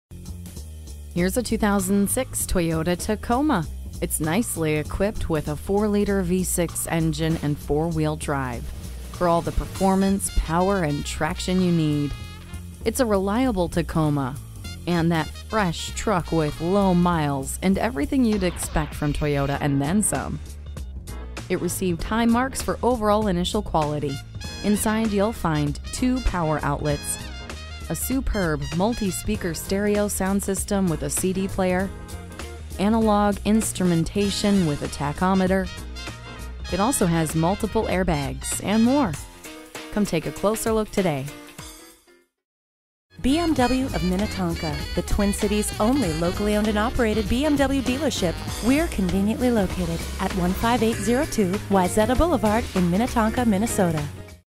Used 2006 Toyota Tacoma Minnetonka MN Minneapolis, MN S36777AA - SOLD
Year: 2006
Make: Toyota
Model: Tacoma
Trim: Access 128 V6 Auto 4WD 4.0L
Engine: 4.0 L V6 Cyl.
Transmission: Automatic
Color: Silver Streak Mica
Interior: Graphite
Mileage: 67955
Stock #: S36777AA
VIN: 5TEUU42N26Z292071
4.0L V6 SMPI DOHC and 4WD. 6 speed manual! Silver Bullet! Come take a look at the deal we have on this terrific, reliable 2006 Toyota Tacoma. This superb Tacoma is the fresh truck with low miles and everything you'd expect from Toyota, and THEN some. J.D. Power and Associates gave the 2006 Tacoma 4 out of 5 Power Circles for Overall Initial Quality.

Address:
15802 Wayzata Blvd